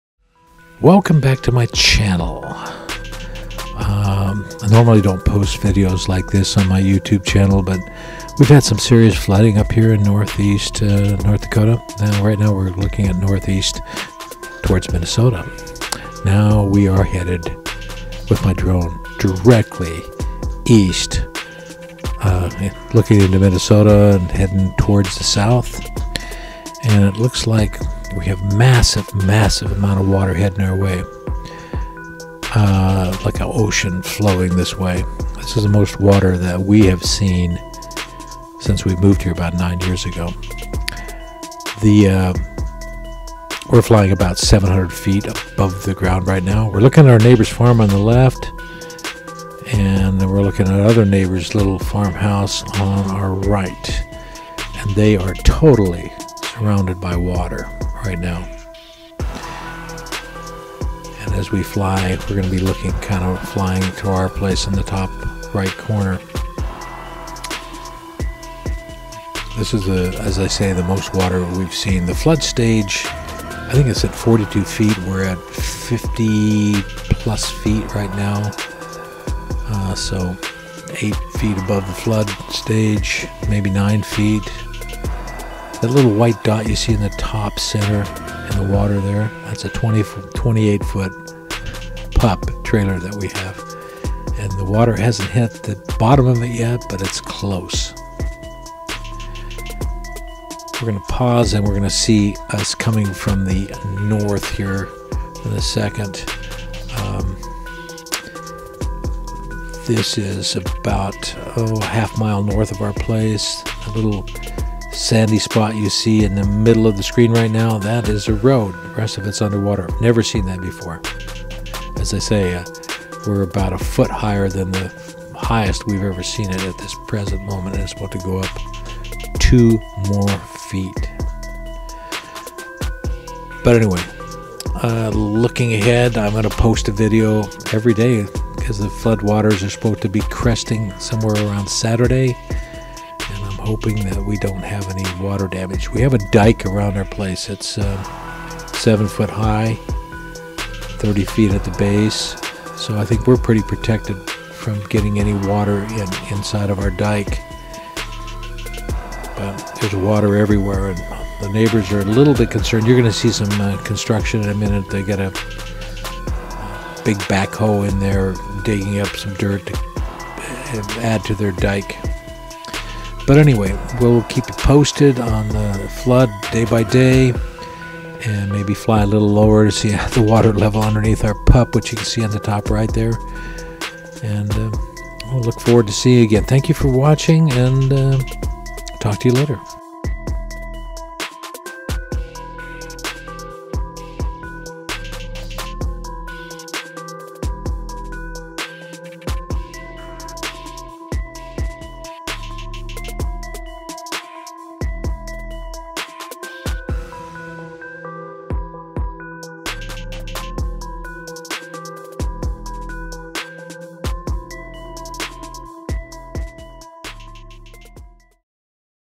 North East North Dakota flooding May 3rd, 2022
This is my first video in a series showing the flood conditions around our home here in North Dakota. The water is rising as I edit this video. The weather service expects at least another 2 feet of flooding over the next few days.